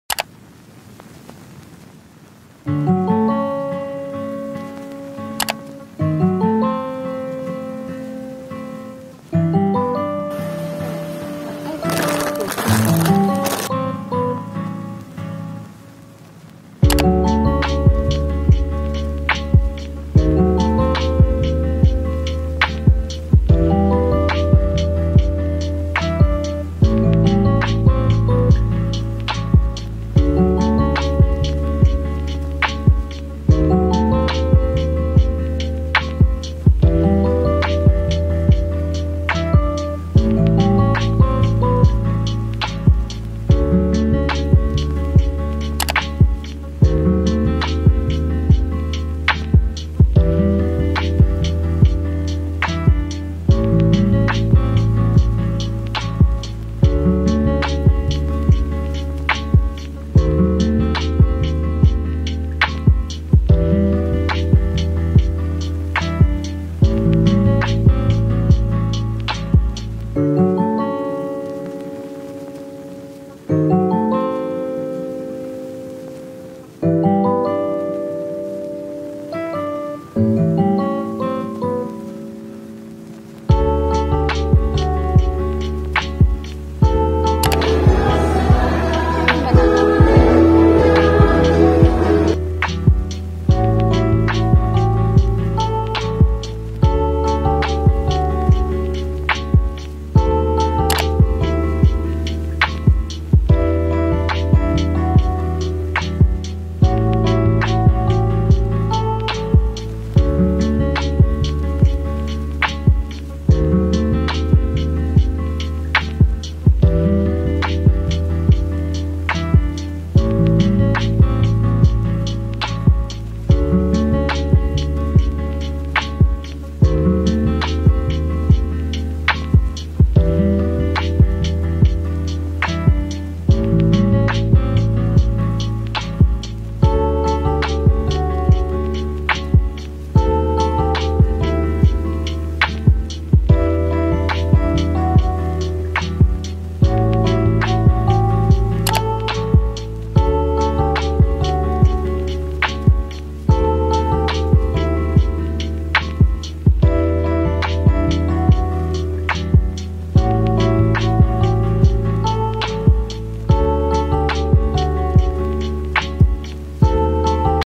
MINI VLOG 1 _ grocery 🛒 new haircut ✂️ church 💒 | CEJ
"Pretend as if you're a vlogger, film random days in your life, and let your closest friends be your only audience."

~ My First Attempt ~